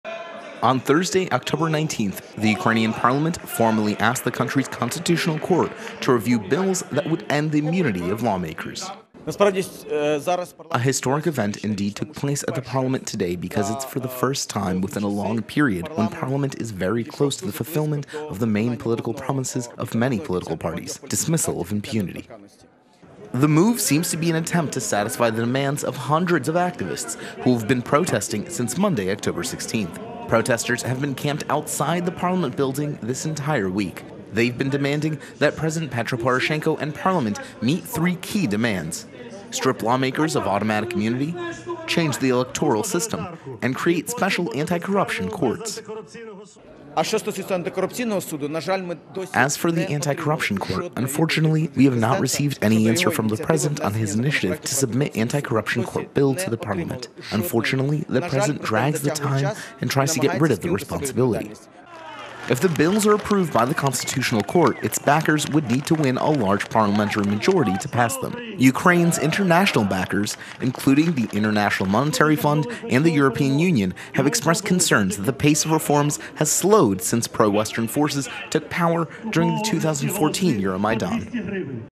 Progress Towards Restricting Parliamentary Immunity
Ukrainian parliament is moving to restrict parliamentary immunity, in a bid to meet protesters' demands. If the bills are approved by the constitutional court, its backers would need to win a large parliamentary majority to pass them.